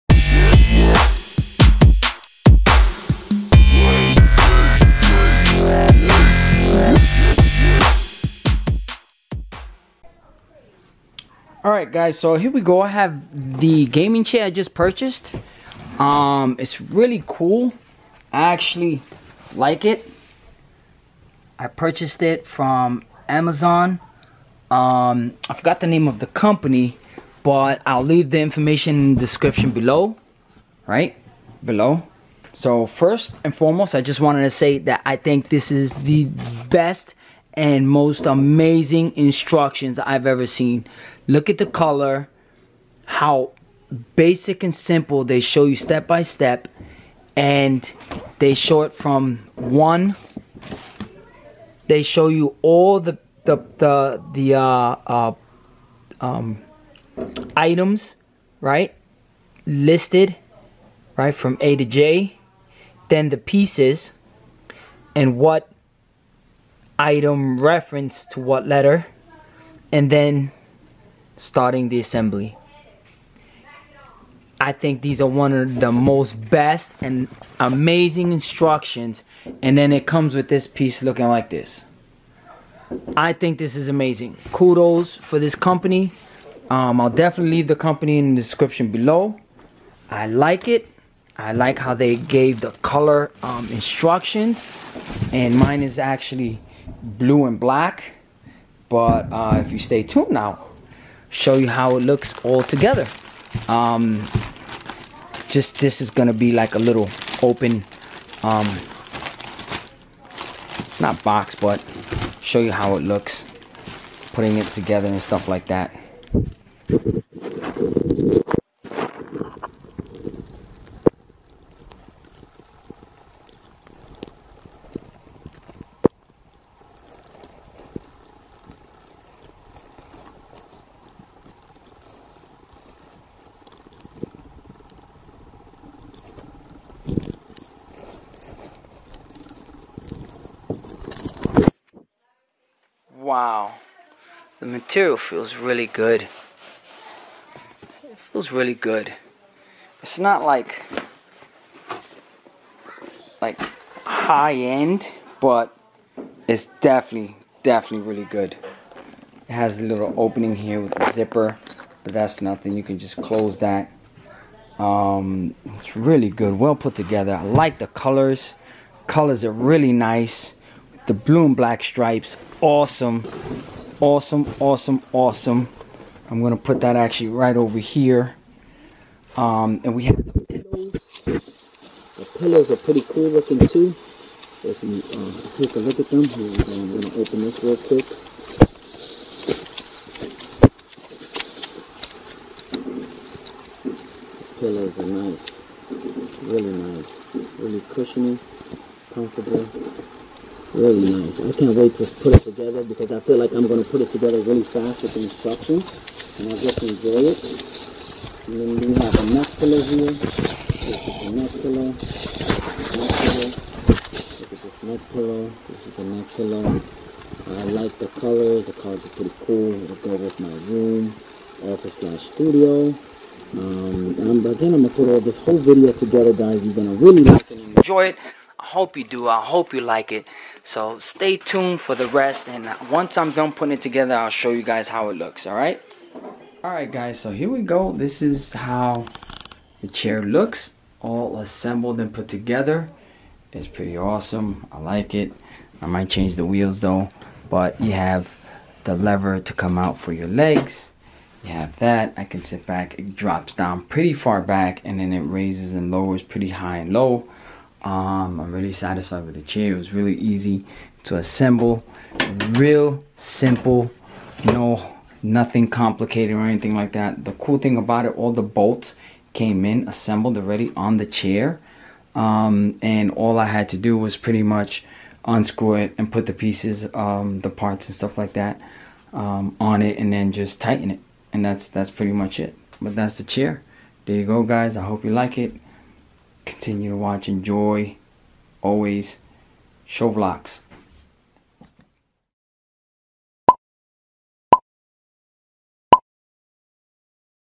Gaming Chair UNBOX | ShowVLOX 🎮🕹🪑
Hey guys welcome back, I have a gaming chair that I purchased from Amazon which is great and just wanted to share it with you all. The chair is blue, black and has a foot rest that slides out. This chair is amazing and is a great addition to my office/studio/game room. I will show it again with the tour of my new room. If your interested in the item there is a link below to where you can view it and or purchase it.

                                                💯***FOLLOW ME***💯

Amazon - Gaming Chair